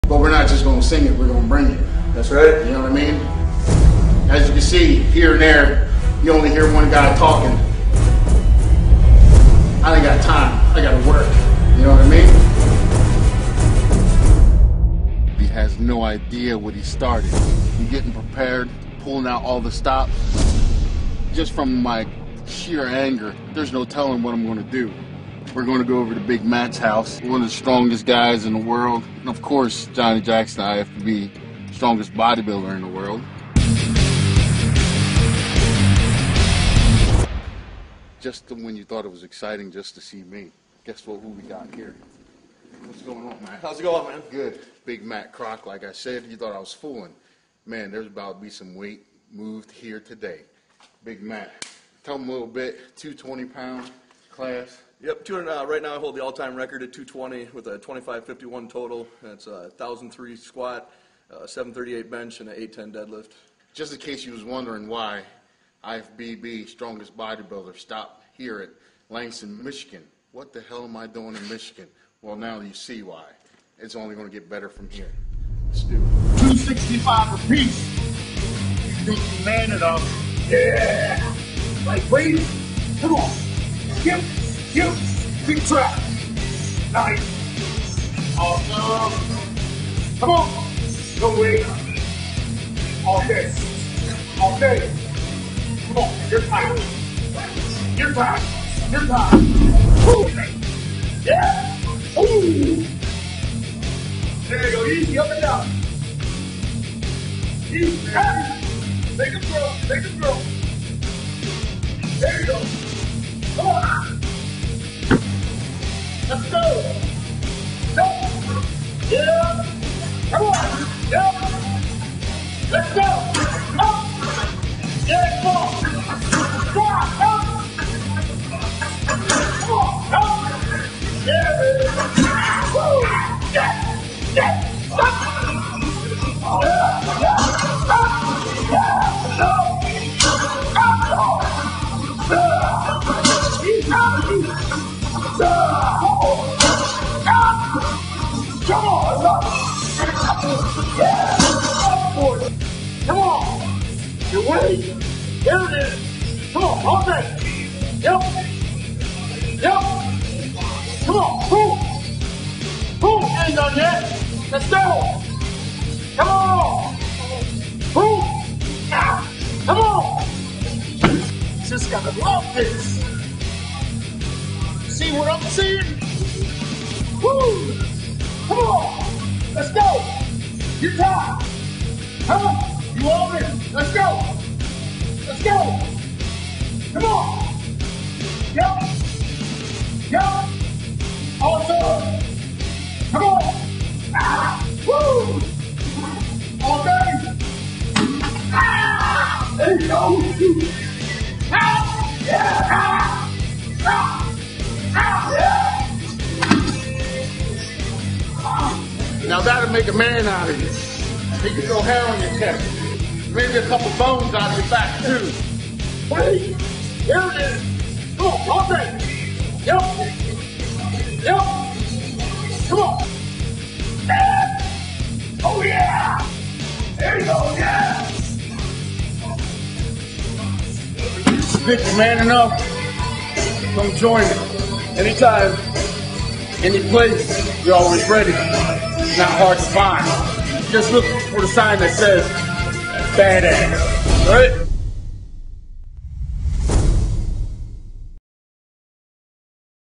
Matt Kroc and Johnnie Jackson powerlifting training
A very old school video of powerlifting legend Matt Kroc and IFBB pro Johnnie Jackson training heavy as Johnnie was preparing for the world’s strongest pro bodybuilder contest at the Olympia.